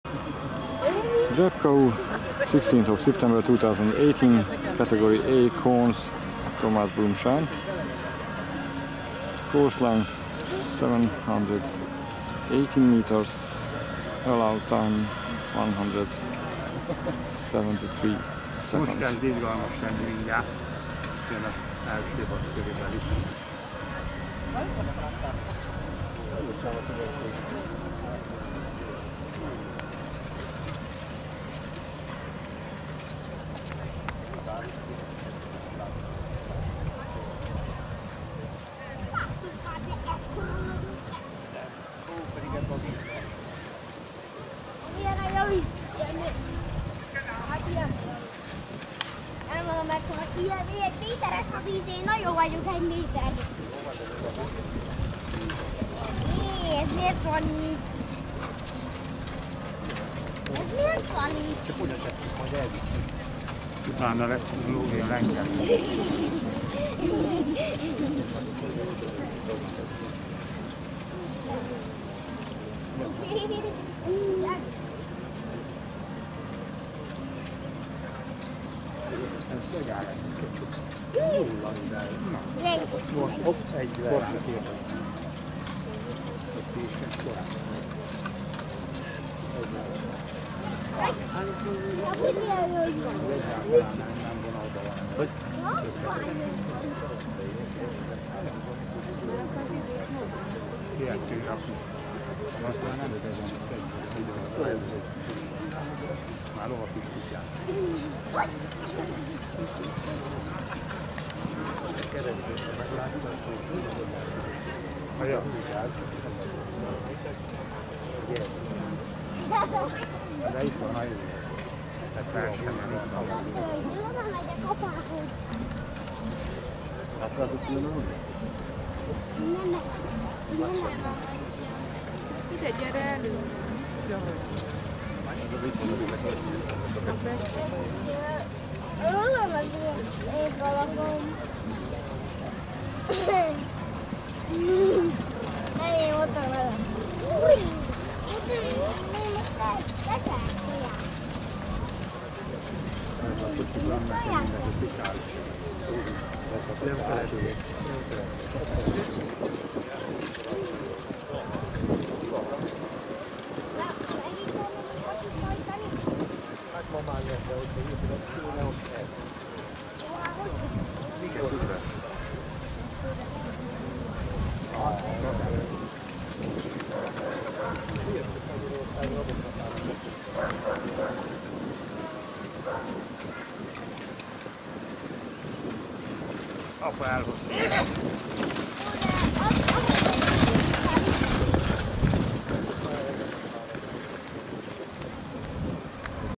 Thomas BLUMSCHEIN (Austria) - CAI-A***, H2, Drebkau (Germany), cones, 16.09.2018
Thomas BLUMSCHEIN (Ausztria) - CAI-A***, H2, Drebkau-Raakow (Németország), akadályhajtás (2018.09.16.)
lovak/horses: 6A: Bailey 3 - 6B: Filko
segédhajtó/groom: ?
pálya hossza/course length: 718 m
alapidő/allowed time: 173 sec